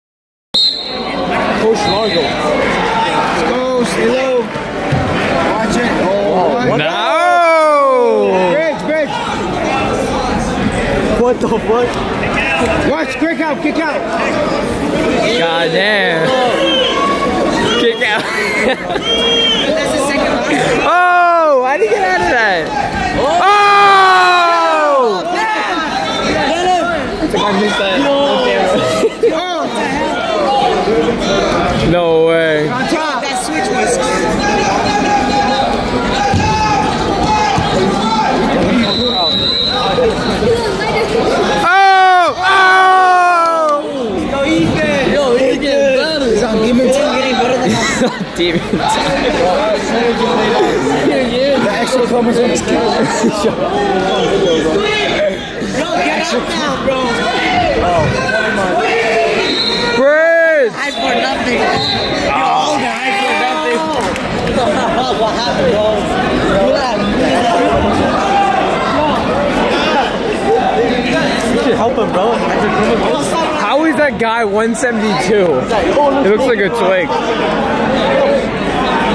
CRAZYEST MATCH OF THE SEASON!!!
this is my match from the last man standing tournament at spring field gardens campus if I'm not wrestling a gold stien kid on Monday then this is my last match of the season